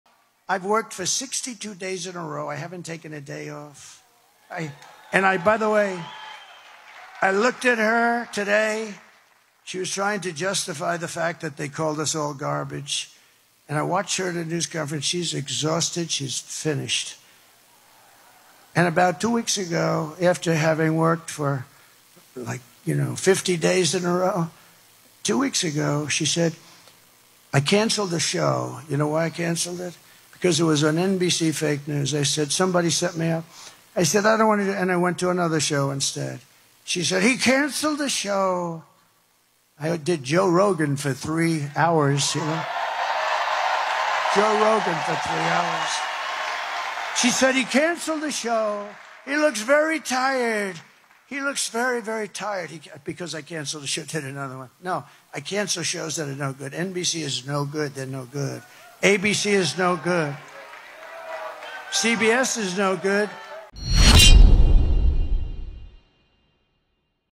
Former President Trump Commented on VP Harris, Saying, 'She's Exhausted, She's Finished'
ANARCHY U' is OUT NOW!